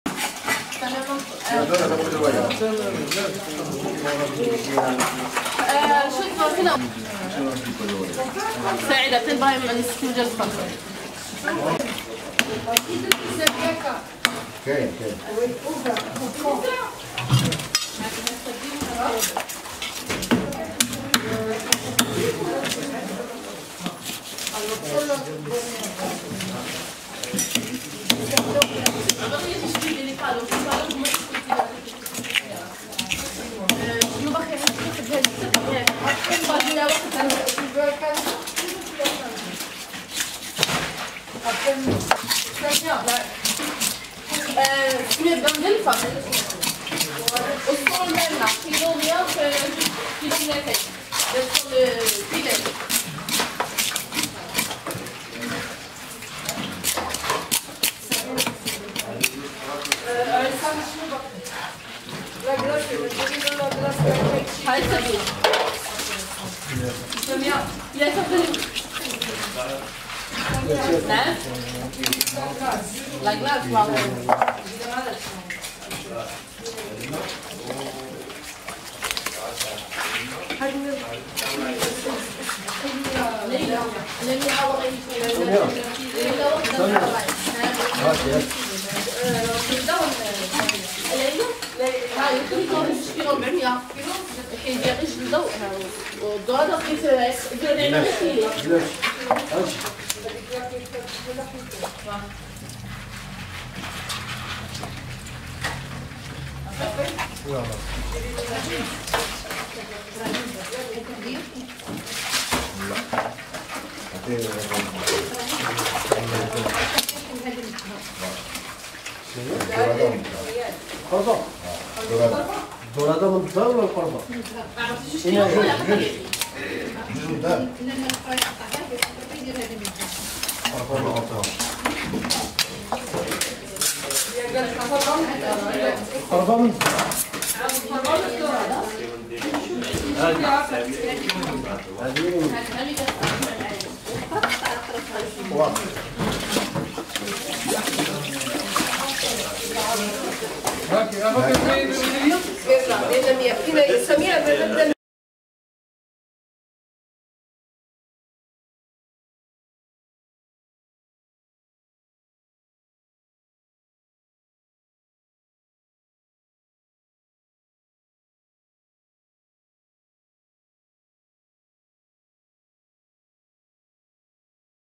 HOW TO CLEAN WHOLE SEA BREAM FISH - FISH SCALING
Red sea-bream
The name of this species is for the large size of the eye, both for the fact that can reach the market with the eyes out of the orbits for the pressure difference undergone at the time of rapid recovery from the steep sea, where it is caught. The eyes of this bream have the iris of bright red. The coloration of the body of young fish is pink, adults have the back color gray golden, silvery on the sides. It is a characteristic a blackish stain at the beginning of the lateral line behind the operculum.
Meat has valued and appreciated for their exquisiteness.